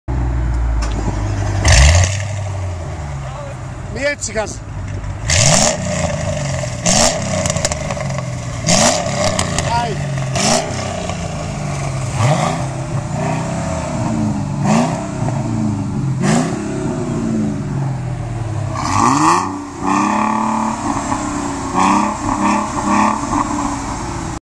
Mercedes-Benz C55, E55, C32 Exhaust sound
C55 Kompressor 610+ HP (non stock)
E55 Kompressor 560+ HP (non stock)
C32 Kompressor 400+HP (non stock)